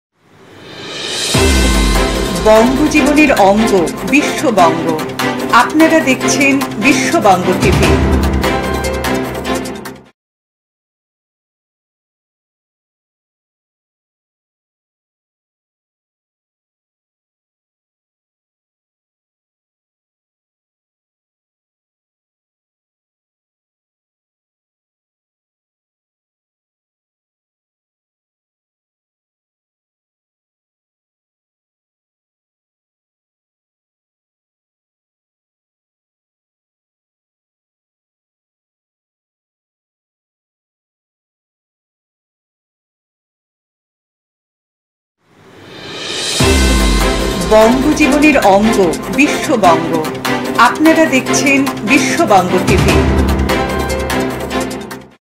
প্রয়াত সমাজসেবী হোমিওপ্যাথি চিকিৎসক জয়ন্ত ব্যানার্জি
প্রয়াত সমাজসেবী হোমিওপ্যাথি চিকিৎসক জয়ন্ত ব্যানার্জি # থাকতেন নিমতায় # তিনি তার মরনোত্তর দেহ দান করে গেছেন সাগর দত্ত মেডিকেল কলেজ হাসপাতালের ডাক্তারি পড়ুয়াদের শিক্ষায়